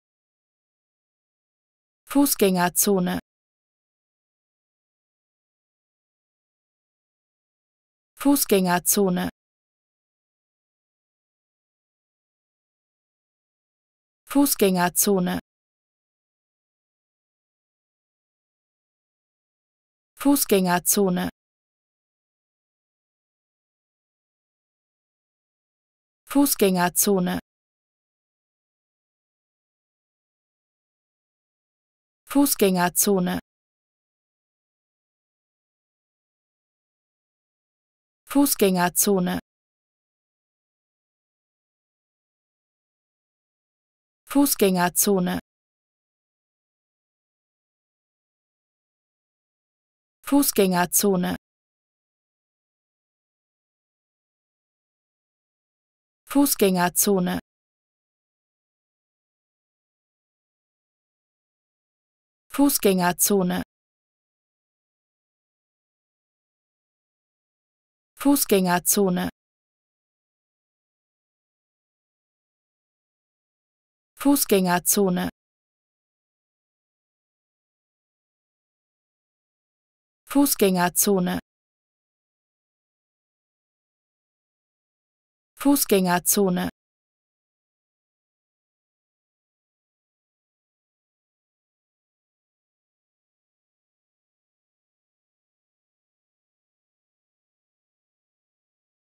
How to Pronounce ''Fußgängerzone'' (Pedestrian Zone) Correctly in German
Learn how to say ''Fußgängerzone'' in German with this short pronunciation tutorial.